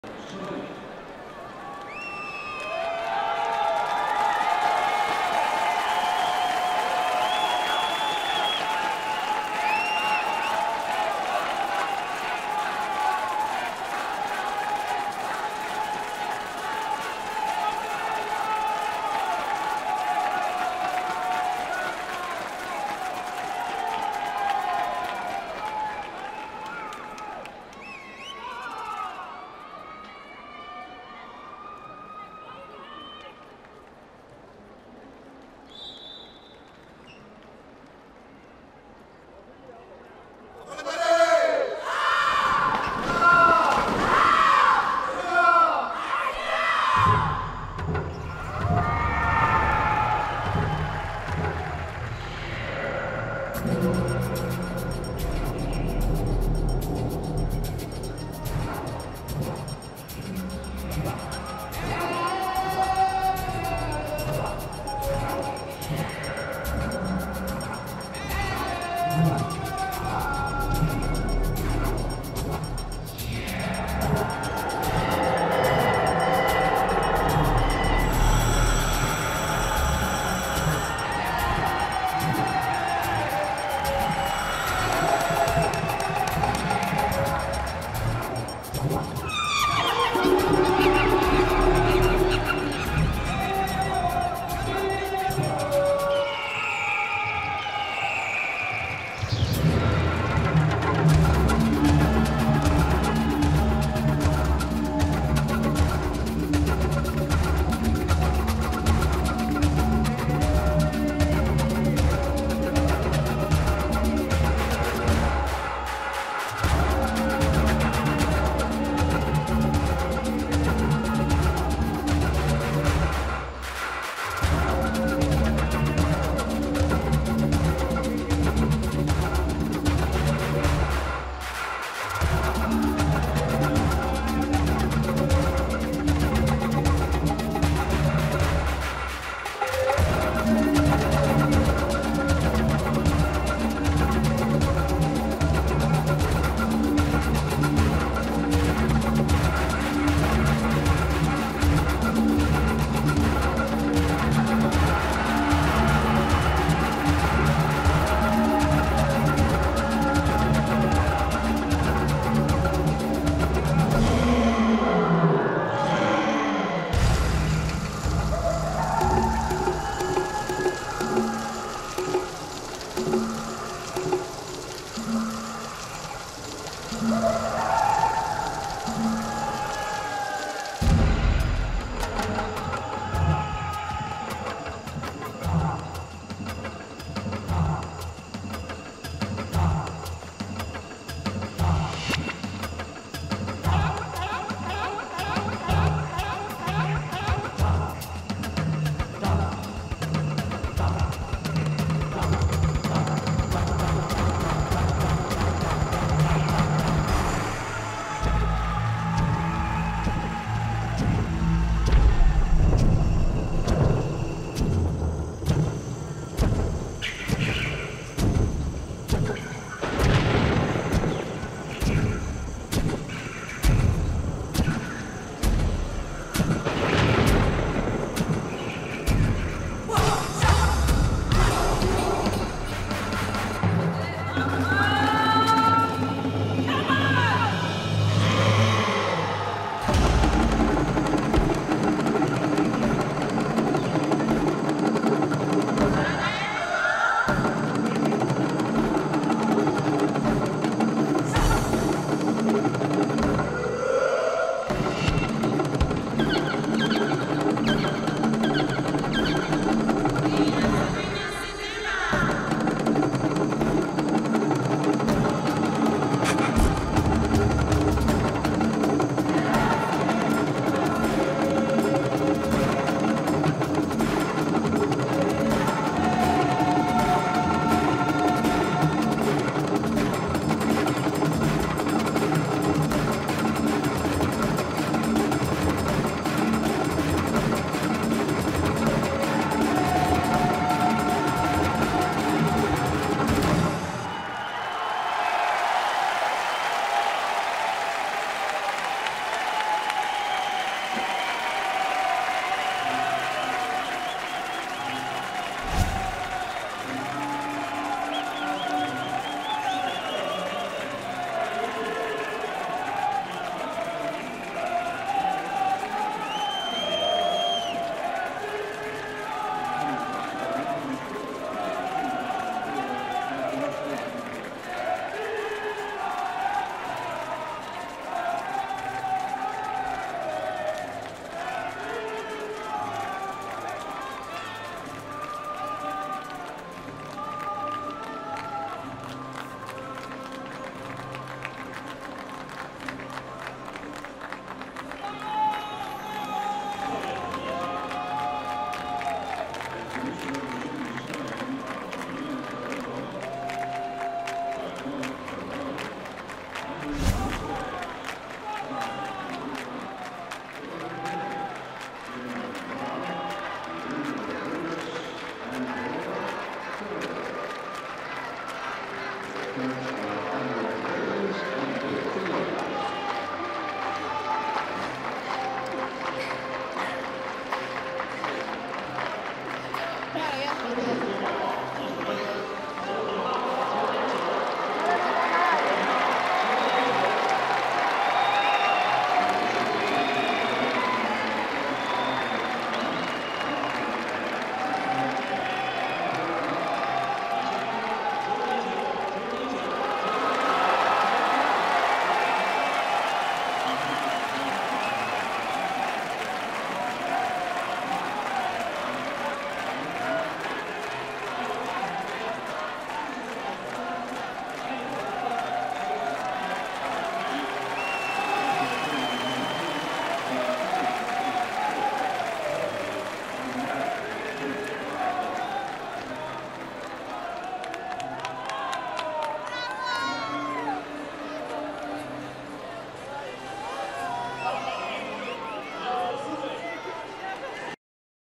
🇪🇸 Team Spain's free routine at Beijing 2008! | FULL LENGTH
We show you Team Spain's artistic swimming free routine at Beijing 2008 in full length! Enjoy watching! Alba Cabello, Raquel Corral, Andrea Fuentes, Thaïs Henríquez, Laura López, Gemma Mengual, Irina Rodríguez, Paola Tirados, and Gisela Morón scored a total of 98.251 points to take the silver medal behind Russia in gold medal position.